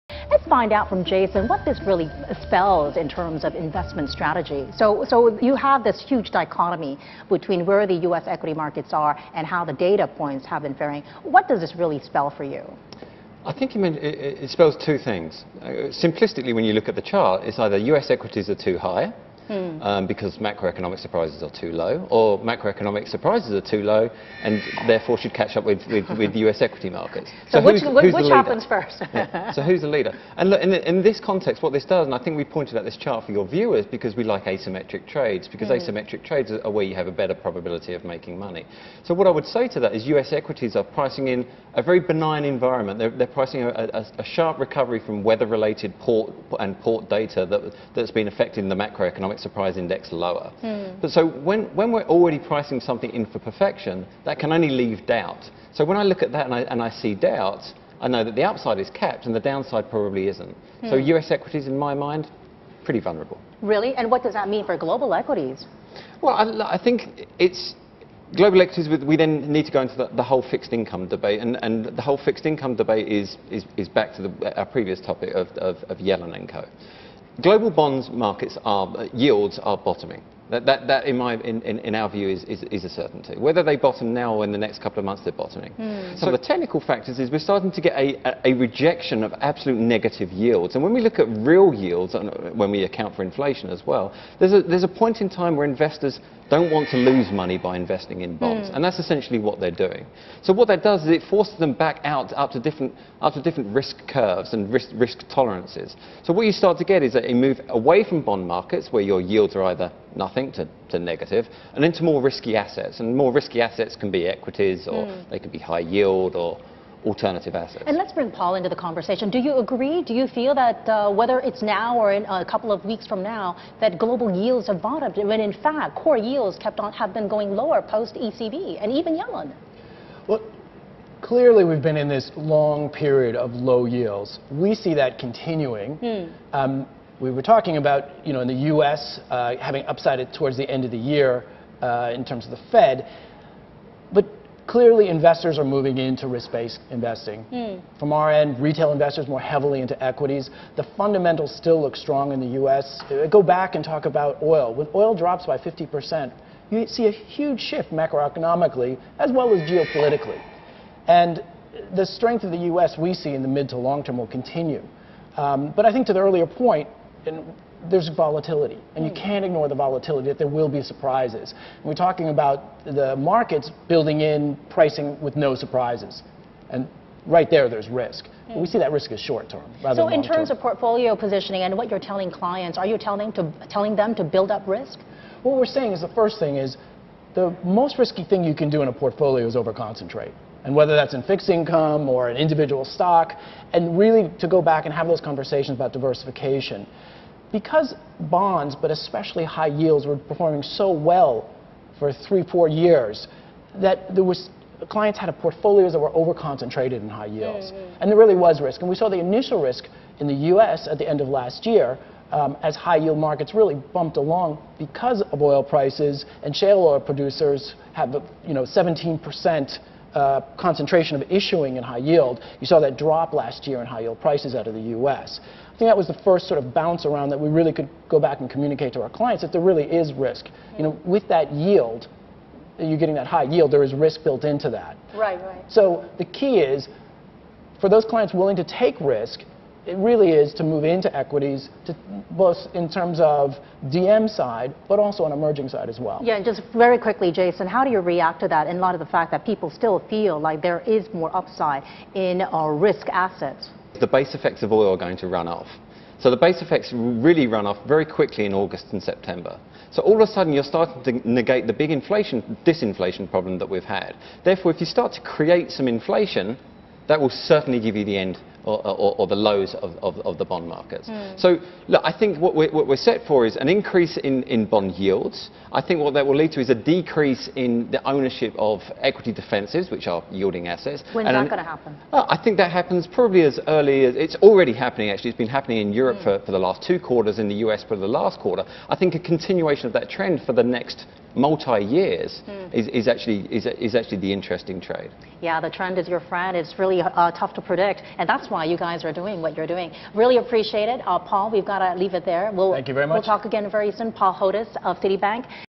INVESTOR INSIGHTS: Global Equities VS U.S Equities
Jason Ambrose, CEO, Vanda Securities  and Paul Hodes, Head of Consumer Wealth Management APAC, Citi debate on the best place for you to put your money.
Find out what they are telling their clients.